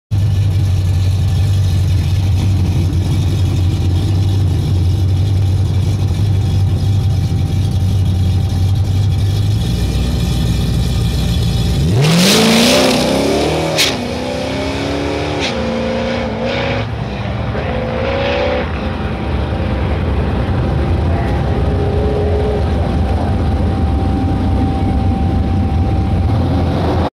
Northeast Dragway+Andys camaro spinning and lifting
Andys camaro at Northeast Dragway spraying a 180 shot of nitrous . 7.71 @ 73.12 MPH